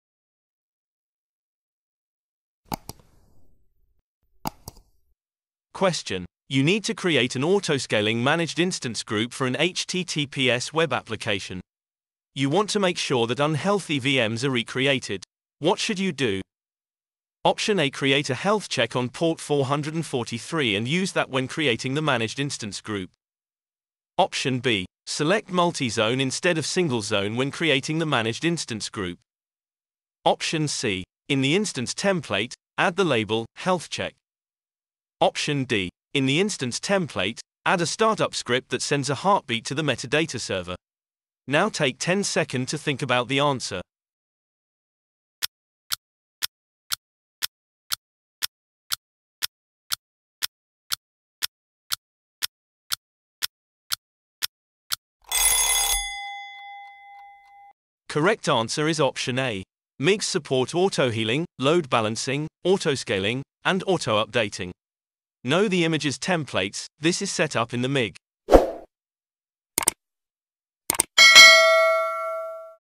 Q51. You need to create an autoscaling managed instance group for an HTTPS web application. You want
Q51. You need to create an autoscaling managed instance group for an HTTPS web application. You want to make sure that unhealthy VMs are recreated. What should you do?

A. Create a health check on port 443 and use that when creating the Managed Instance Group.
B. Select Multi-Zone instead of Single-Zone when creating the Managed Instance Group.
C. In the Instance Template, add the label 'health-check'.
D. In the Instance Template, add a startup script that sends a heartbeat to the metadata server.